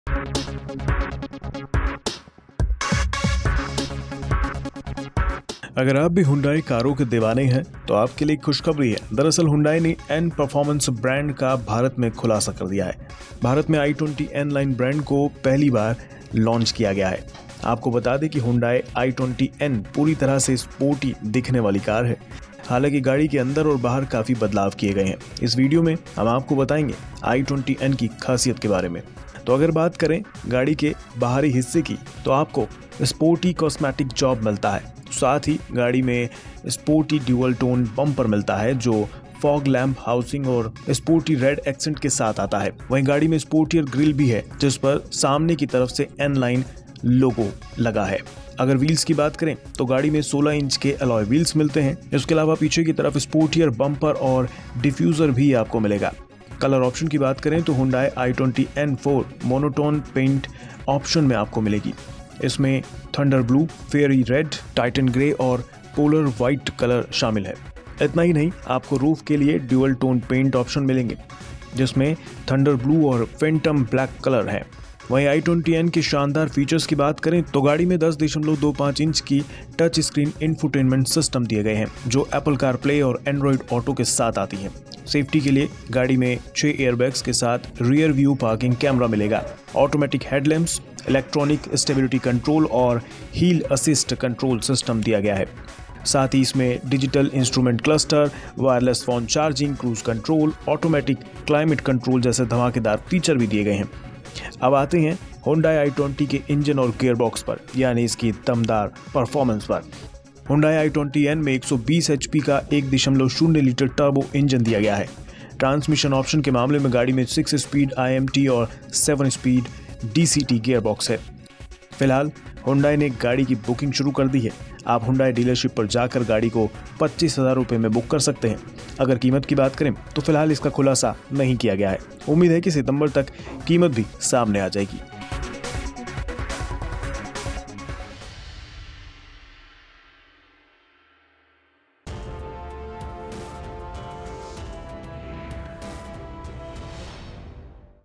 Hyundai i20 N लाइन का भारत में हुआ खुलासा, मिलते हैं कई दमदार फीचर्स | BizTak
अगर आप हुंडई की कारों के दिवाने हैं तो आपके लिए एक खुशखबरी है... दरअसल हुंडई ने N परफॉर्मेंस ब्रैंड का भारत में खुलासा कर दिया है. भारत में i20 N लाइन ब्रैंड को पहली बार लॉन्च किया गया है. आपको बताते दें कि हुंडई i20 N पूरी तरह से स्पोर्टी दिखने वाली कार है.